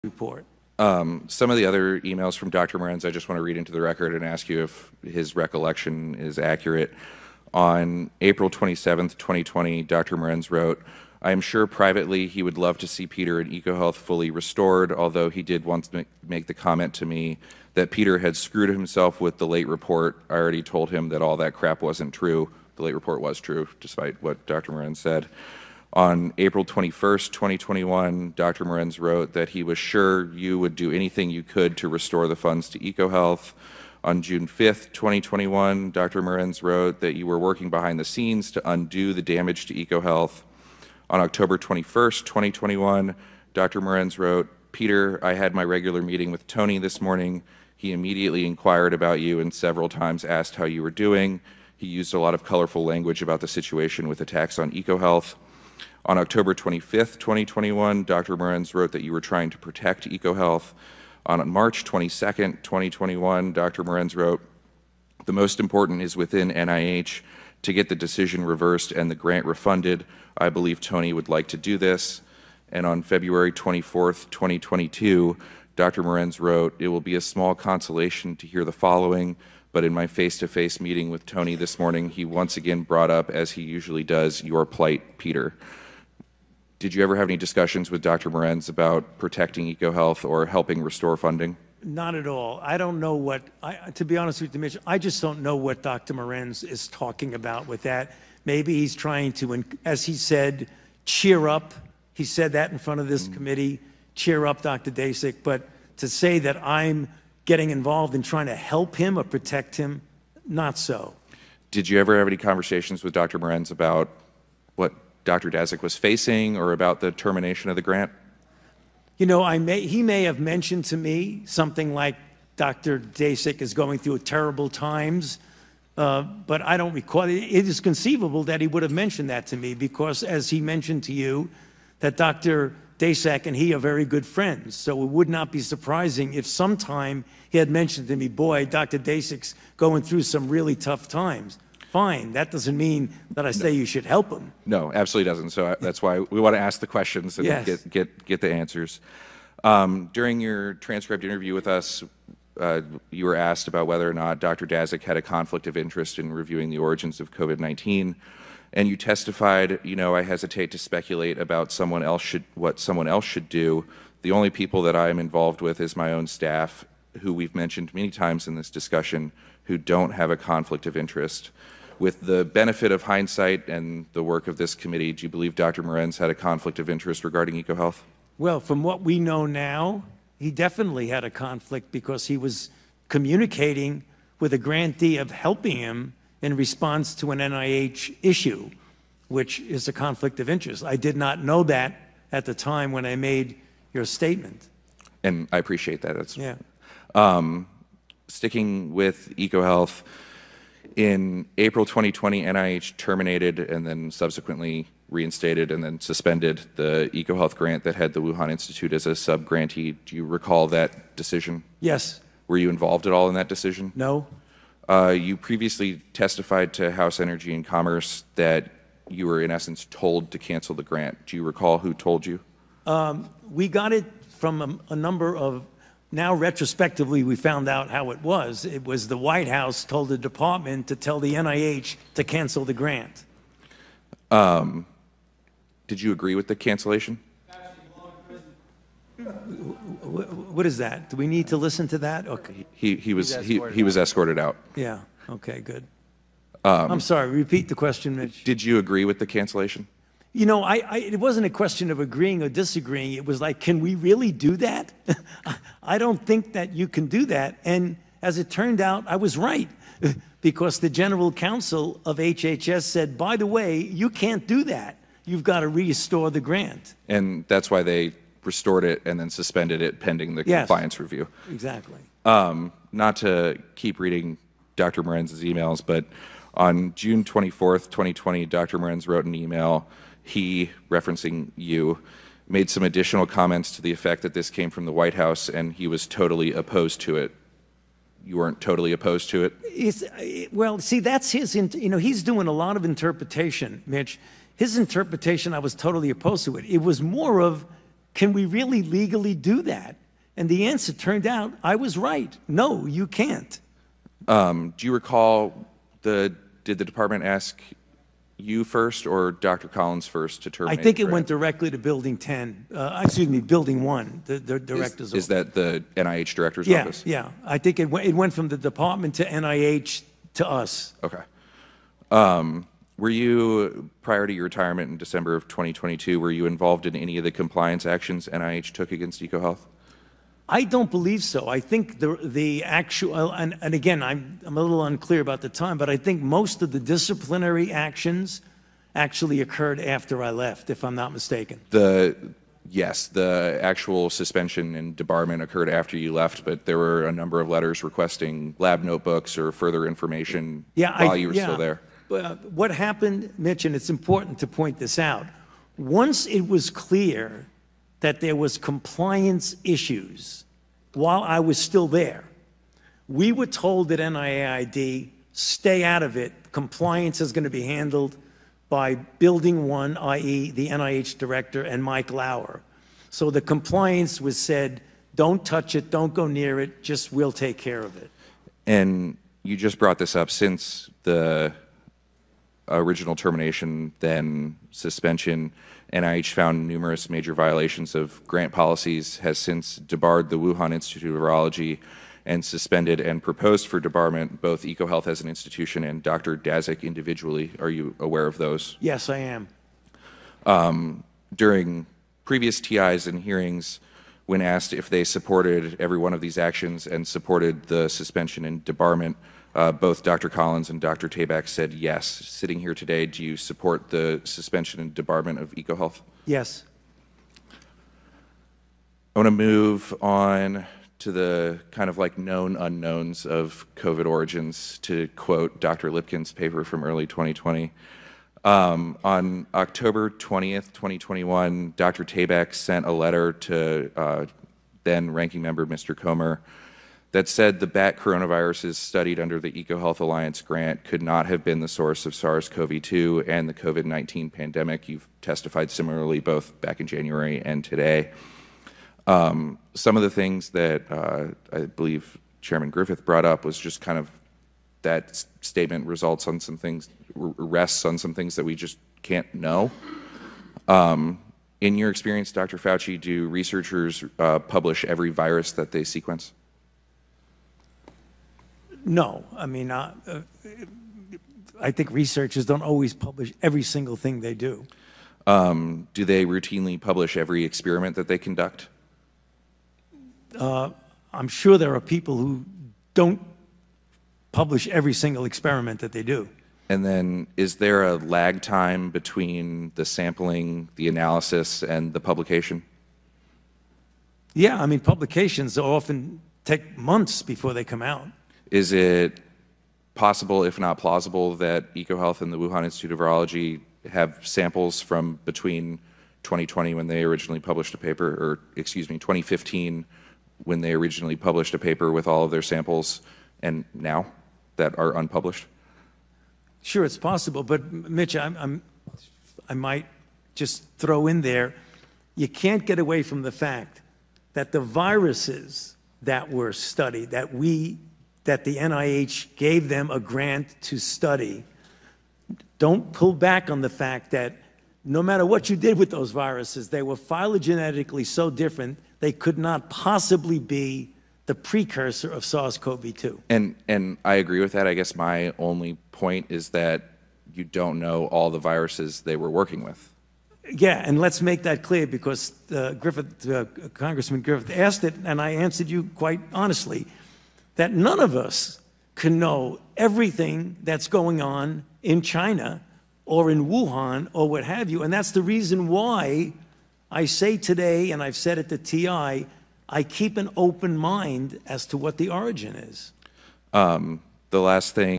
‘Did You Agree With The Cancellation?’: GOP’s Lawyer Grills Dr. Fauci On Ecohealth Funding
At last week's House Select Committee on the Coronavirus, the GOP majority's lawyer Mitchell Benzine questioned Dr. Anthony Fauci about funding for the EcoHealth Alliance.